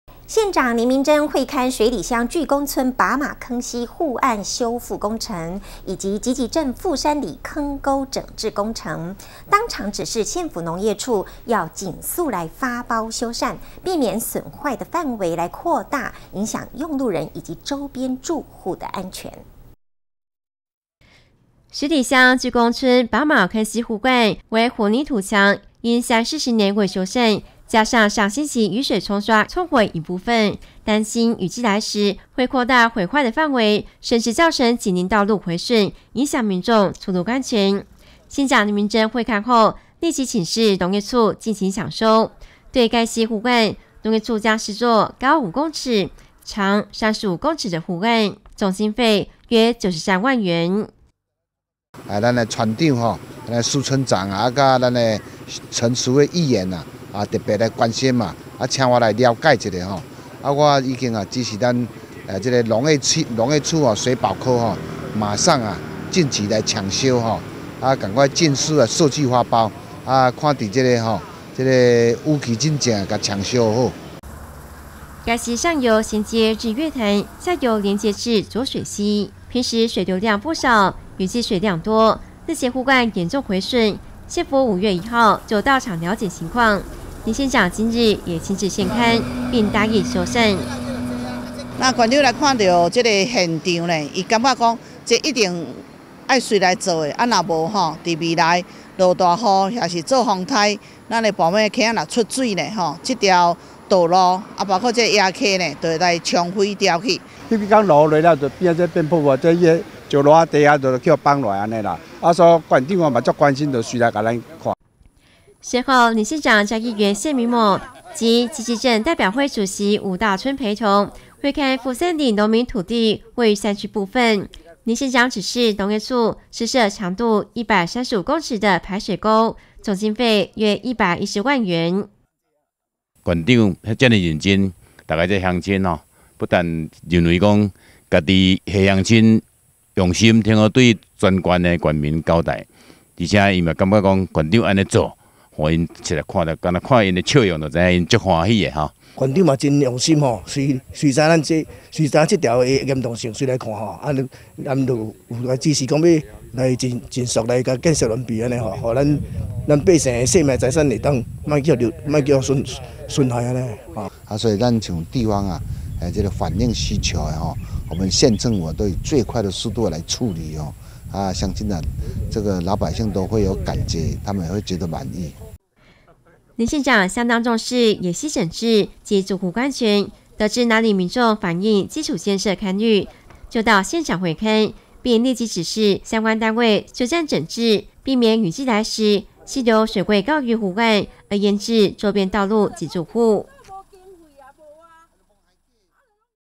水里及集集護岸坍塌 縣長指示儘速搶修
縣長林明溱會勘水里鄉鉅工村拔馬坑溪護岸修復工程與集集鎮富山里坑溝整治工程，當場指示縣府農業處儘速發包修繕，避免損壞範圍擴大，影響用路人及周邊住戶安全。